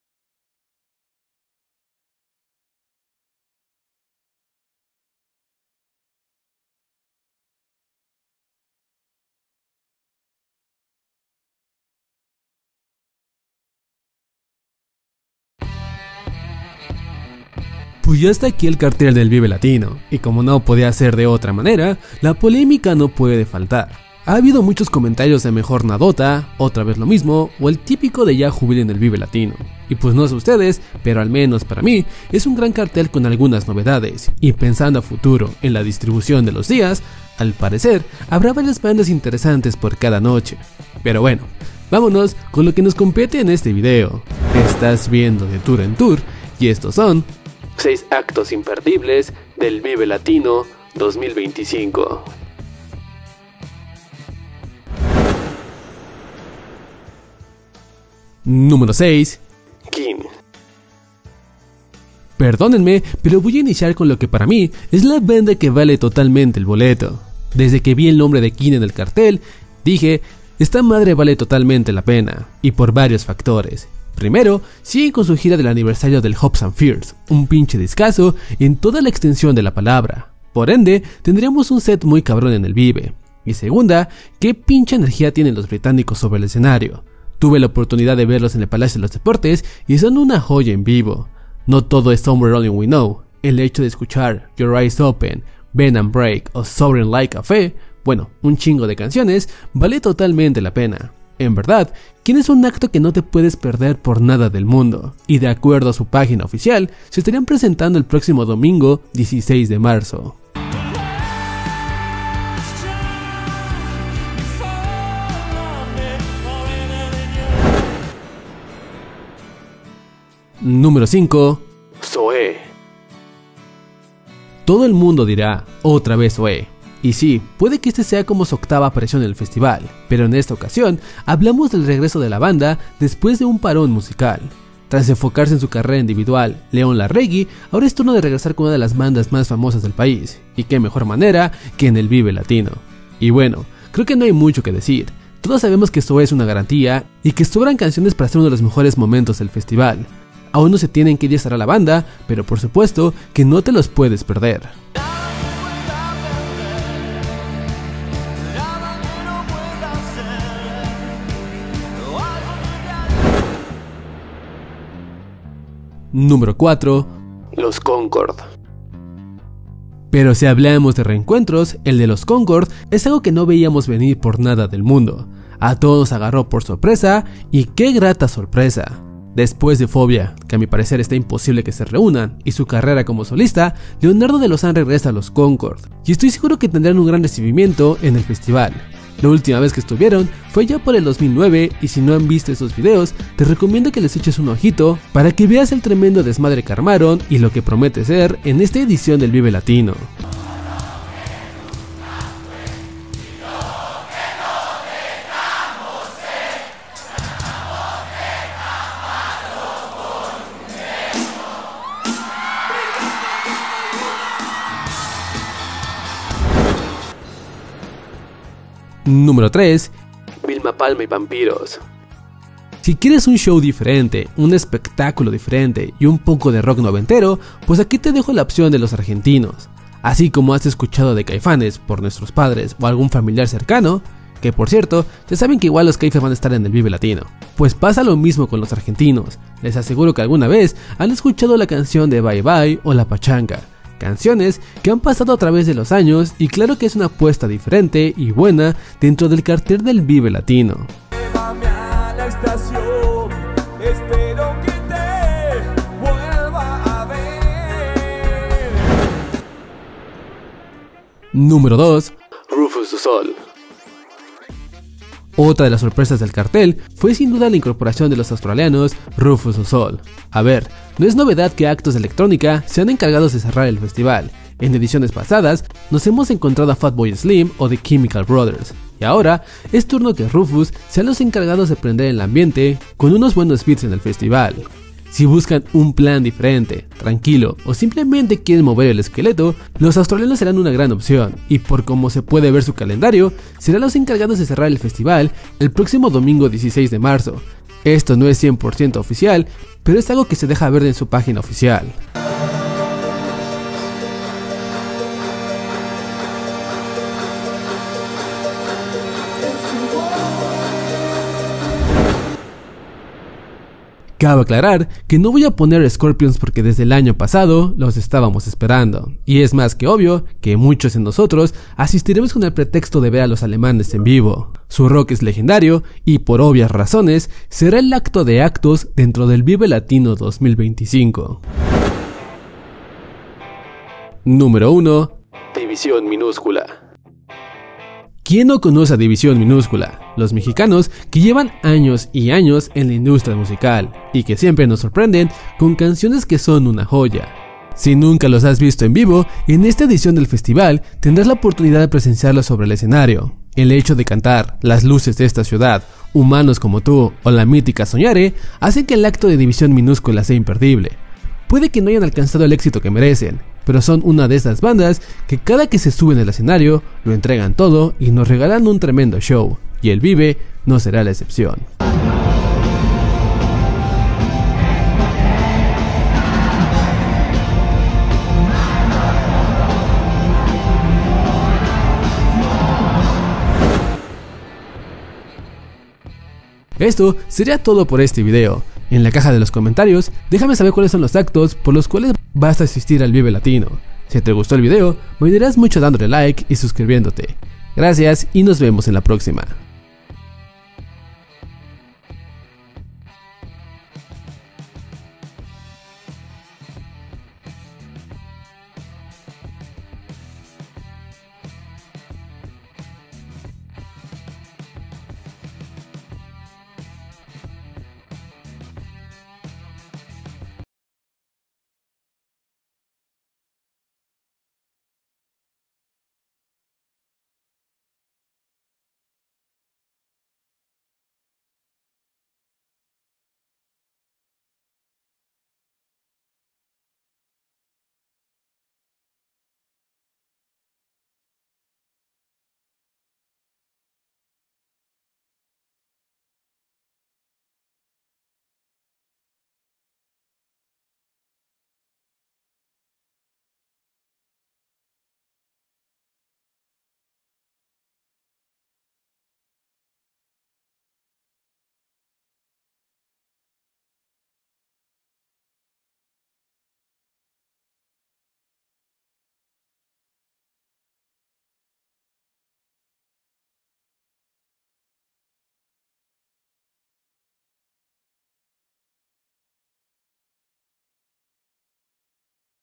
¡6 Bandas IMPERDIBLES del Vive Latino 2025!
En esta ocasión, hablamos de 6 actos imperdibles del vive latino 2025, sabemos que faltan varios por mencionar por lo que en la caja de los comentarios, déjame saber por cual acto asistirás a esta edición.